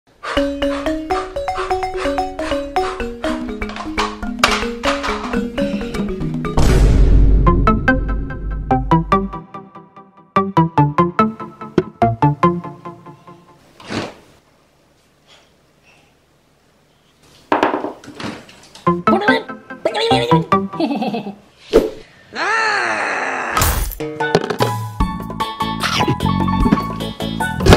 TRAINING OR FOOD
Sometimes training is better than eating. However, today I decided on donuts from the refrigerator. Reasons You’re Not Losing Weight
Ponekad je bolje trenirati nego večerati. Ipak danas sam se odlučio za krofne iz frizidera.